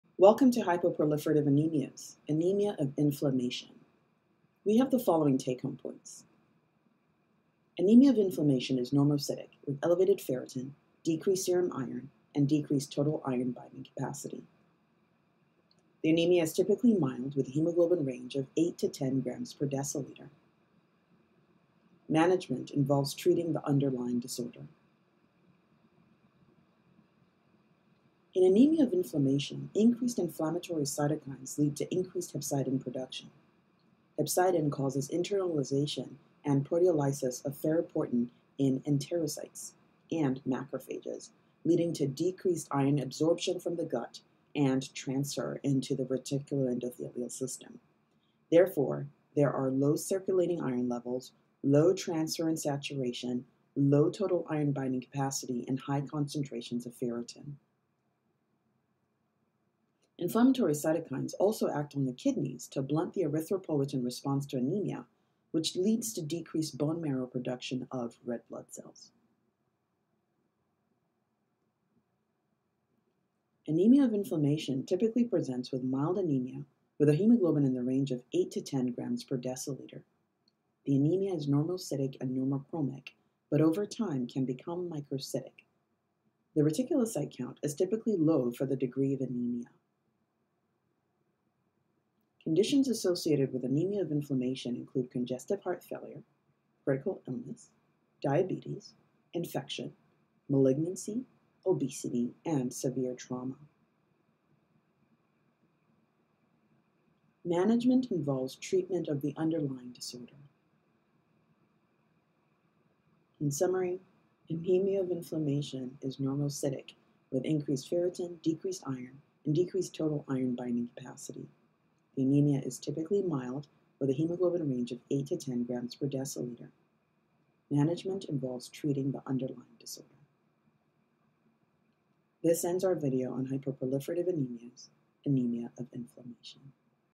Anemia of Inflammation
Also called anemia of chronic disease, anemia of inflammation is an important cause of anemia in both the outpatient and inpatient setting. Inflammatory cytokines lead to increased hepcidin production and decreased erythropoietin response to anemia. The result is a mild normocytic and normochromic anemia with increased ferritin.